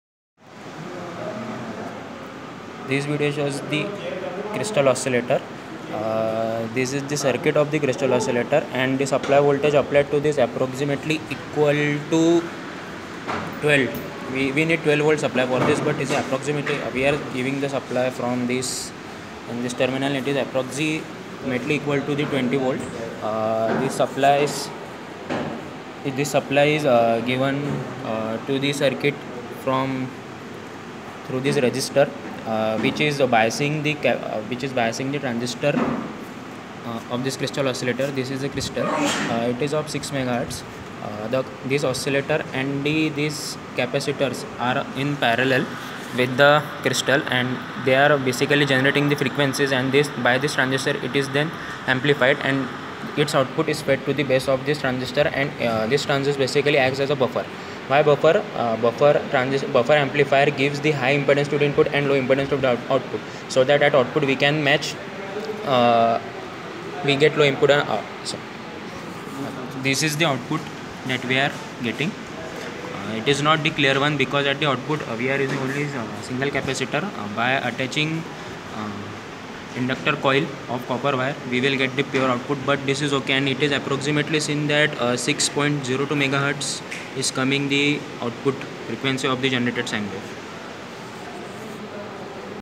6 MHz Crystal Oscillator
Mini Project Designed by: Sumit Patil, Arihant Dhobe, Rushab Jain, Shivam Tripathi (Fourth Sem. Students)